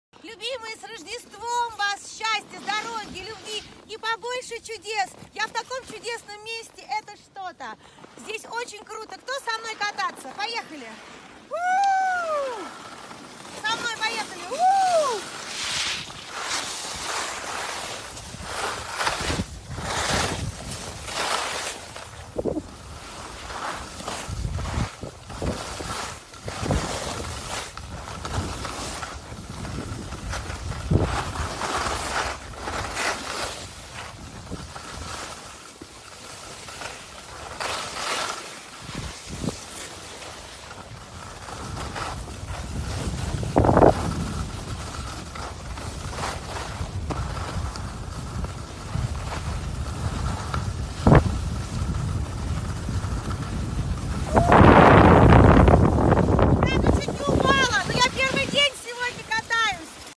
Анна Семенович катается на лыжах
Певица Анна Семенович решила расслабиться после тяжелой трудовой недели. Артистка пела и плясала на различных корпоративах, выступала